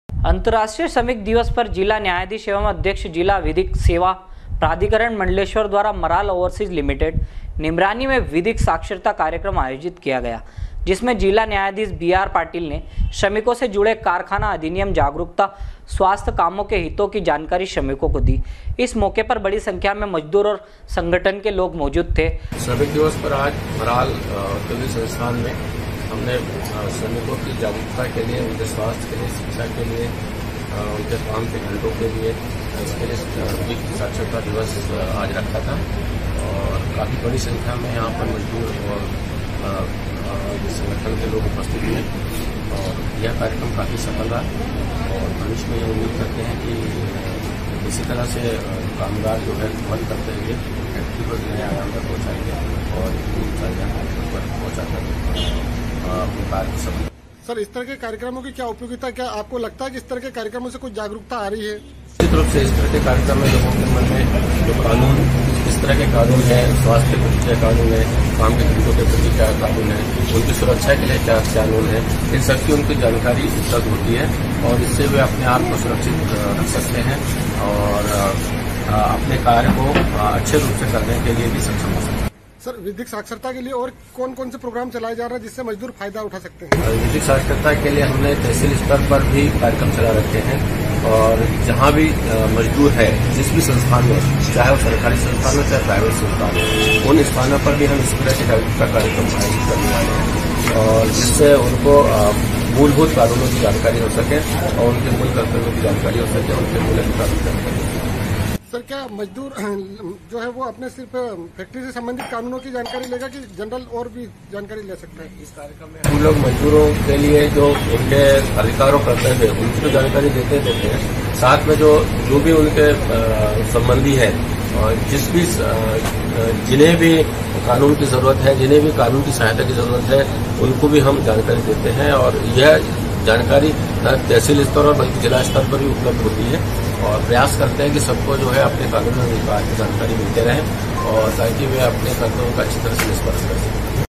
sw24news kasrawad , 01.05.2019
अंतर्राष्ट्रीय श्रमिक दिवस पर जिला न्यायाधीष एवं अध्यक्ष जिला विधिक सेवा प्राधिकरण मण्डलेष्वर द्वारा मराल ओवरसिज लिमिटेड, निमरानी में विधिक साक्षरता कार्यक्रम आयोजित किया गया । जिसमें जिला न्यायाधीष बी आर पाटील ने श्रमिको से जुडे कारखाना अधिनियम, जागरुकता, स्वास्थ्य, कामो के हितो की जानकारी श्रमिको को दी । इस मौके पर बड़ी संख्या में मजदूरो और संगठन के लोगो मौजूद थे